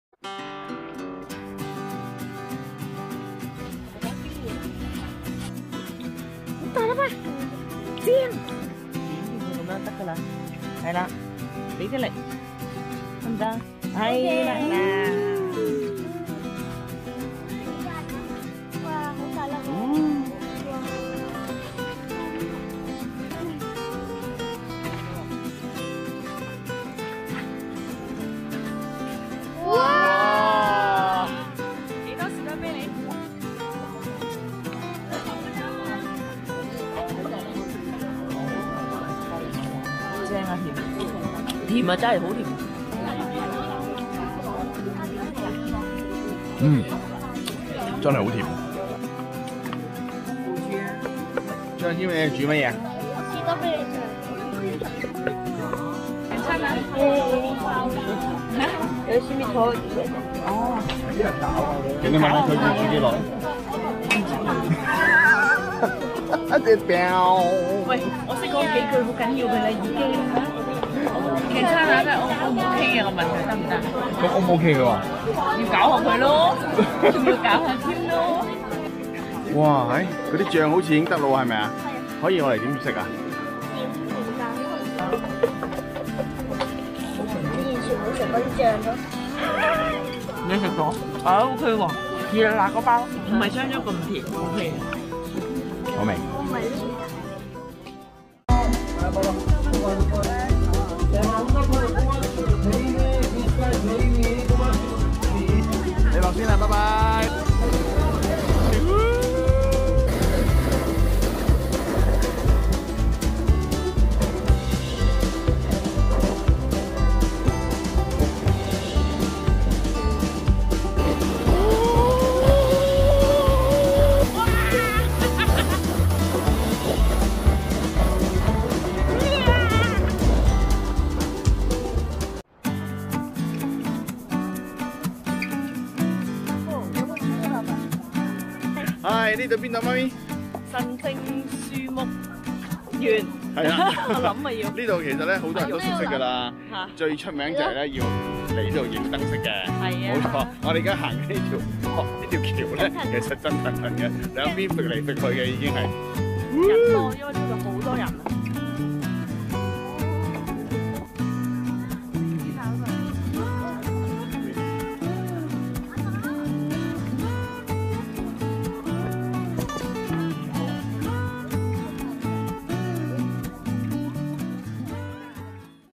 快靚正一日團
一早買定One day tour 嘅好處係唔使諗, 有得玩有得食, 可以去遠啲😍 摘完士多啤梨, 仲可以整果醬, 之後去玩雪兜  夜晚仲去埋睇燈飾。